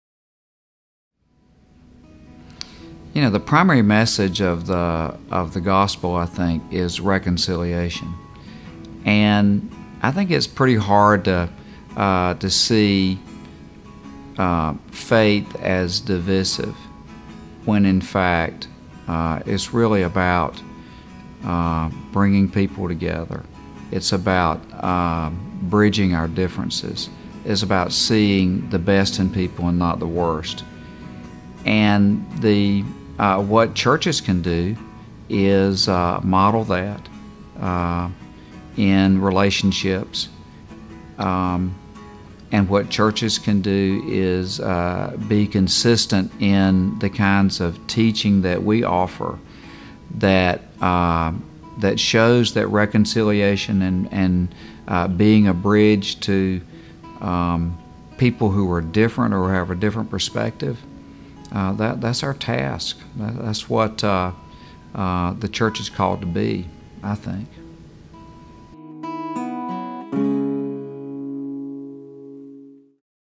Sid Batts: Reconciliation
Sid Batts, senior pastor at First Presbyterian Church in Greensboro, N.C., discusses churches' ability to bridge gaps and bring people with differences together.